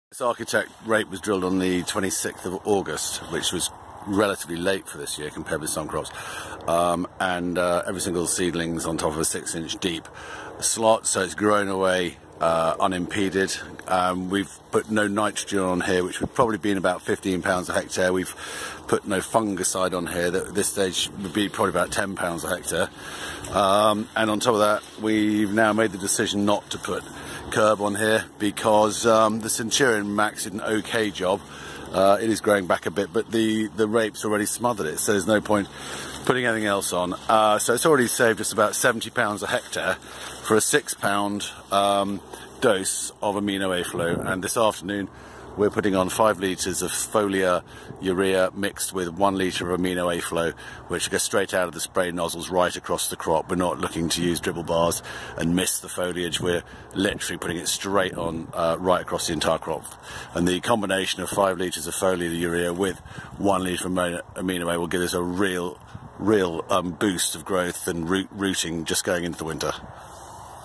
Architect OSR about to receive second dose AminoA FLO and foliar urea.
Architect OSR direct drilled with Triton seed drill on wide row spacing. No autumn nitrogen applied about to achieve full gound cover. Just about to recive second dose of AminoA FLO with 5kg foliar nitrogen to maintain crop vigour going in to the Winter. This is a very low cost crop that the grower claims has saved £60 ha over his typical growing costs at this stage.